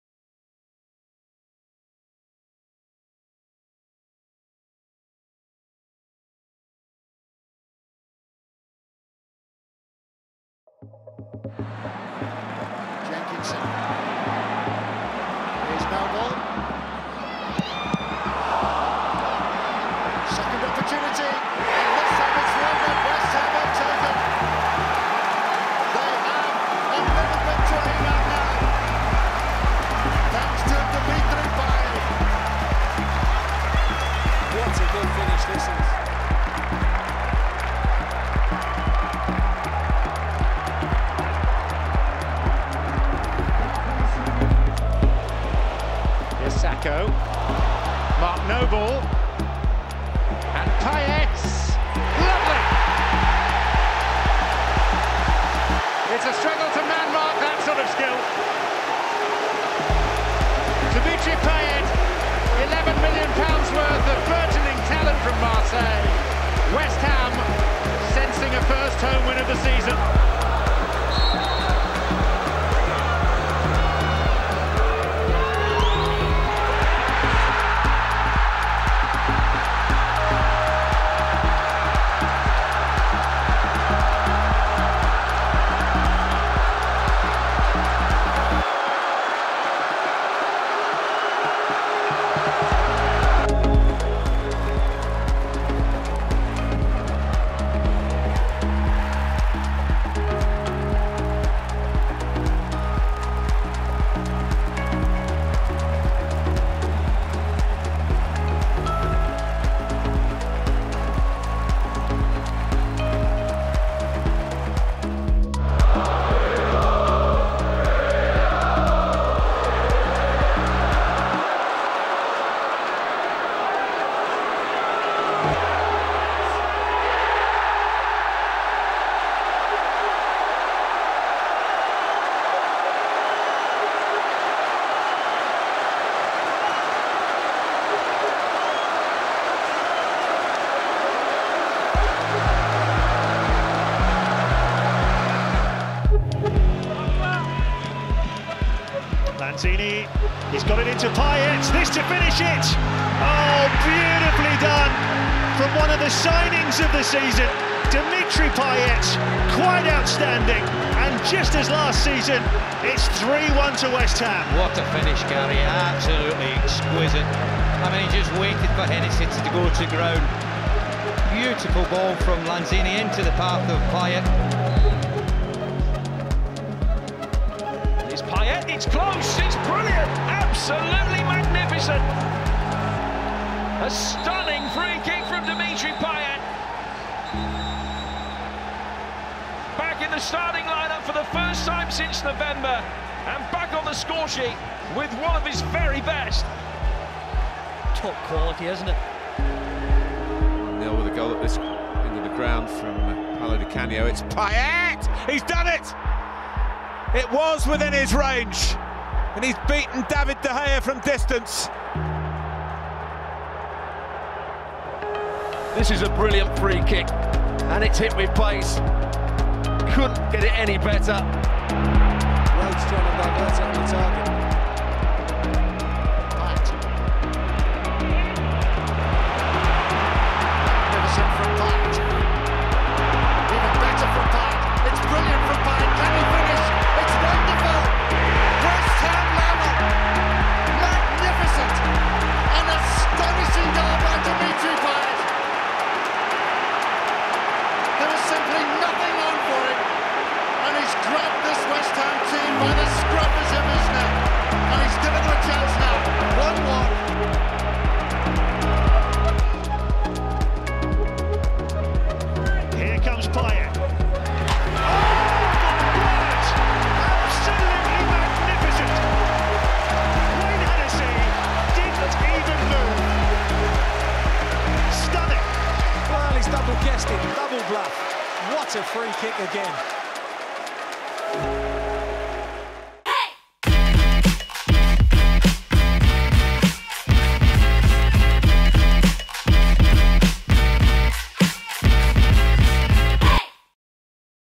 Unstoppable Freekicks, Cheeky Chips & More | Dimitri Payet's Top 10 West Ham Goals ⚒️
Take a look back at Dimitri Payet's top 10 goals for the Hammers. Let us know which one was your favourite.